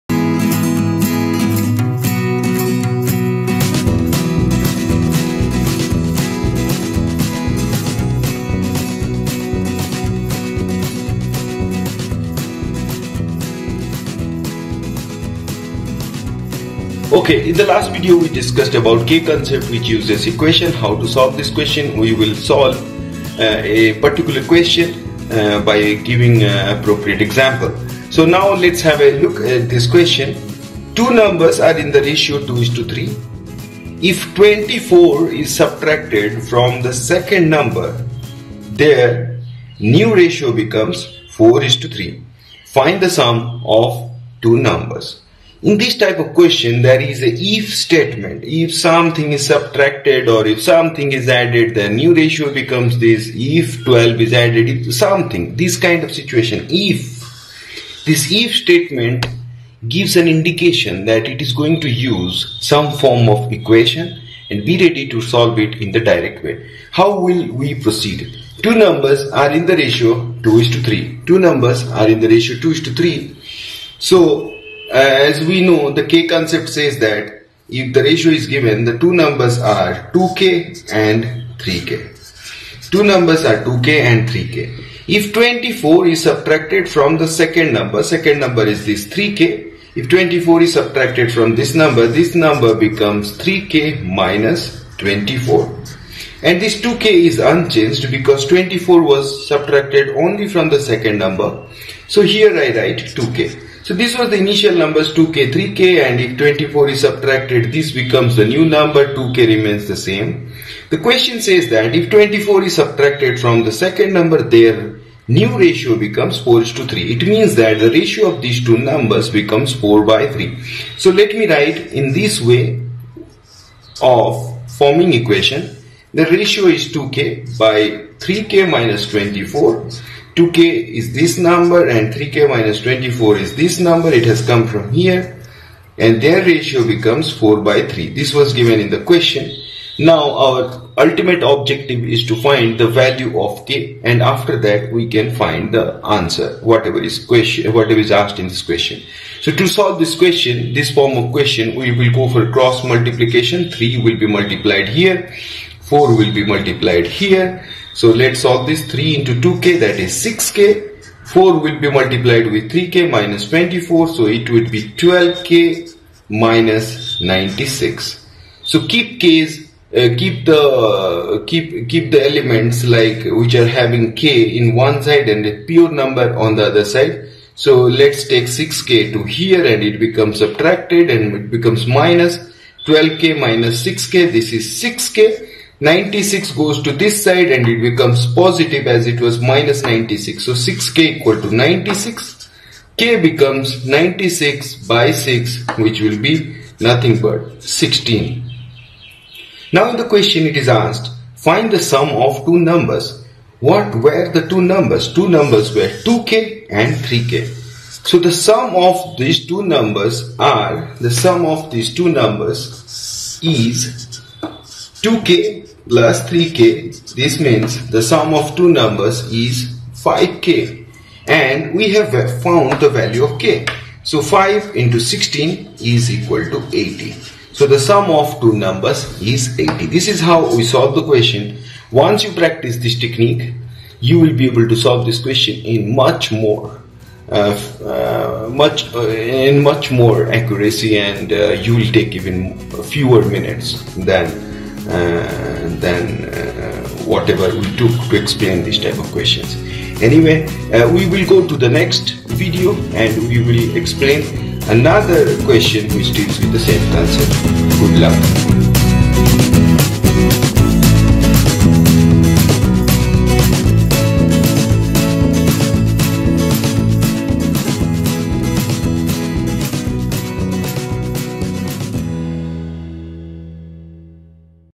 Video#9 CONDITIONAL RATIO-very useful for IBPS/SSC
This video explains you the tricks involved in solving ratio based questions, which use a condition in it. Ensure that you understand how k concept is used and an equation is framed to solve a conditional question.

You must learn how cross multiplication is getting used.

We learnt k concept. Now with this video, we will learn how promptly we can use this concept to solve complex questions. Be smart and solve it quick.

Most of times in different competitive examination, these type of questions come with different weightage. So learn it well so that it would be easy to implement the concept quite spontaneously.

When we mention the ratio as a mathematical tool, many students fail to realize it. This video will explain you what a ratio is. This concept of ratio which is literally known as k concept is used in further chapters like DATA INTERPRETATION and TIME AND WORK.
And moreover, the concepts involved in ratio is highly useful for various examination like IBPS-PO/CLERK, SSC, RAILWAY, IBPS-SO SIDBI, CAT/MAT, Railways , LIC, IB examination.
Good Luck.